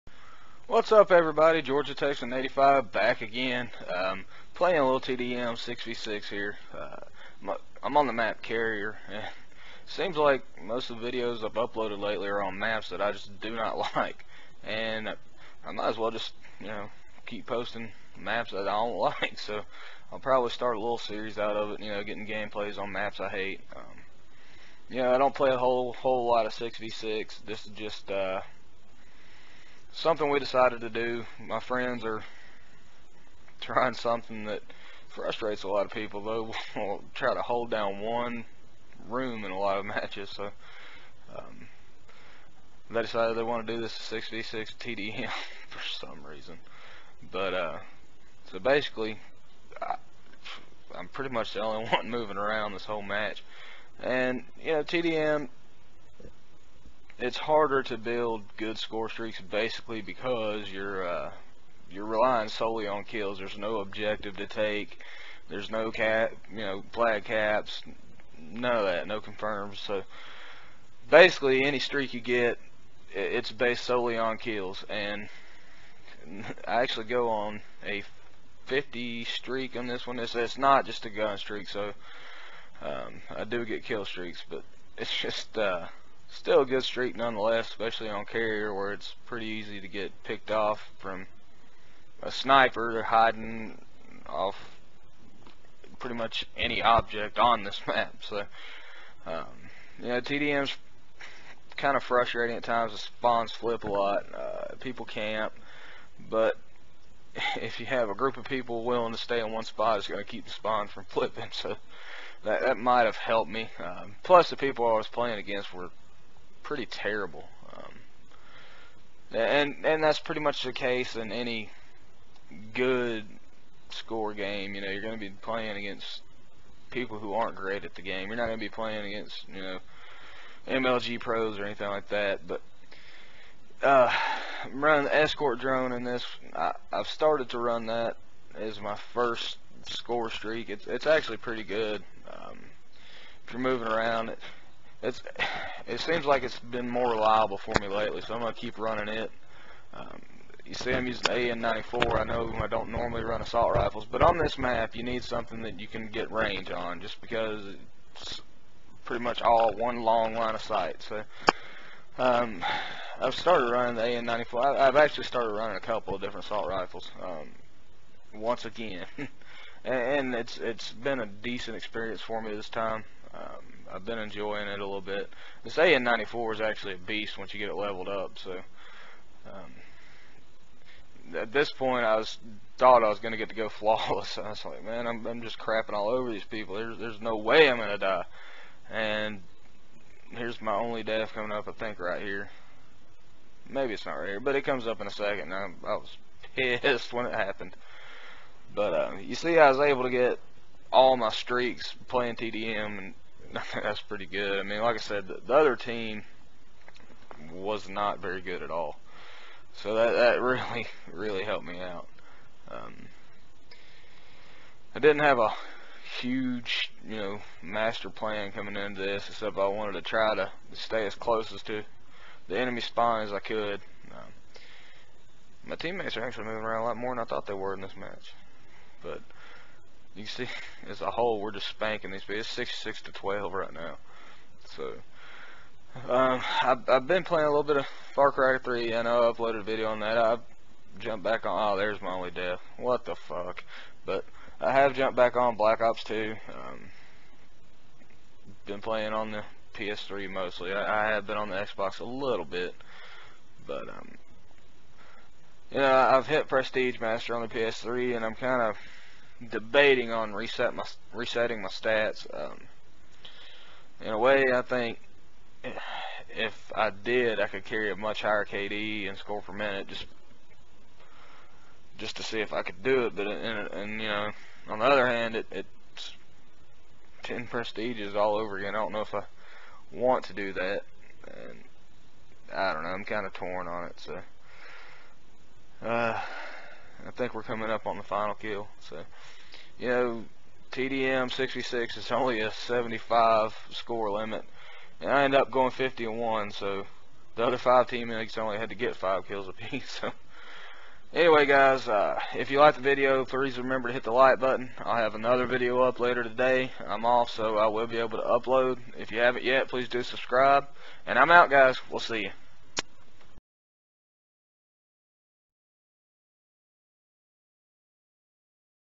Maps I hate Ep. 1... Should I reset my stats?
50-1 playing 6v6 TDM on Carrier.